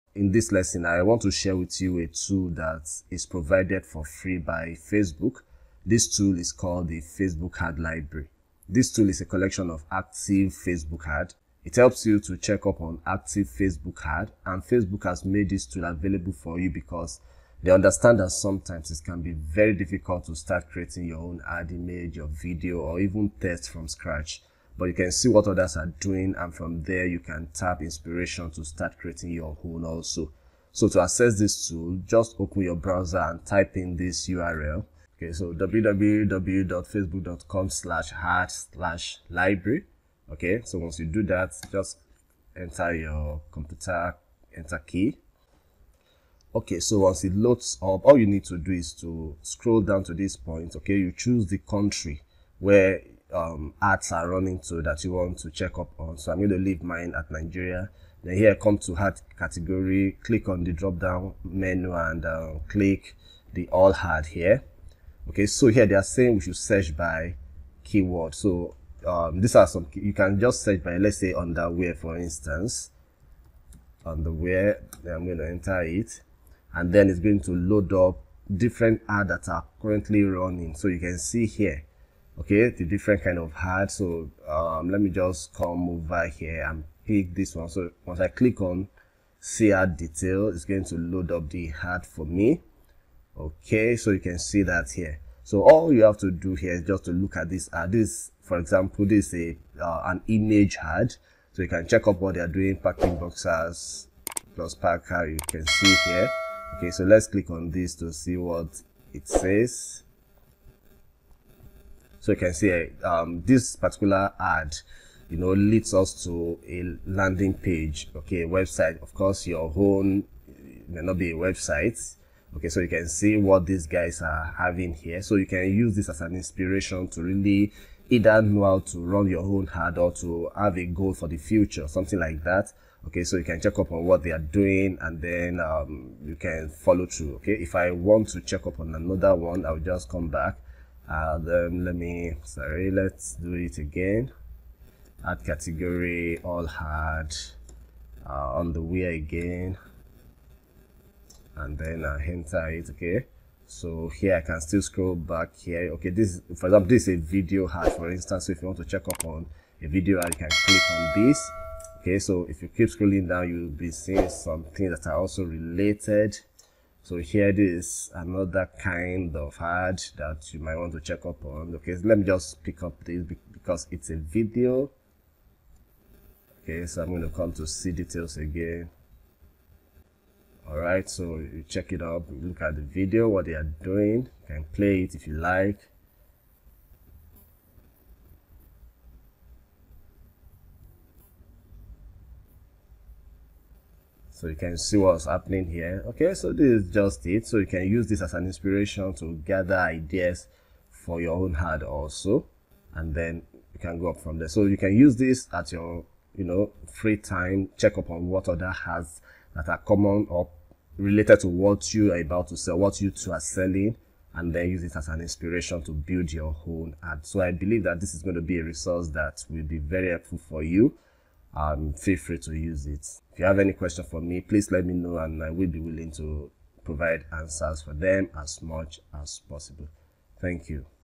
Hidden Facebook Ad Library Tricks For Crafting Winning Ads! (1,000% Ad Result Performance In 3 MINS)
This video shows you how to use Facebook ad library, a free Facebook ad tool that lets you spy on top active Facebook ads so that you can get inspiration to craft your own winning ads.
Use the tricks I shared to learn how to use Facebook ad library to find winning products.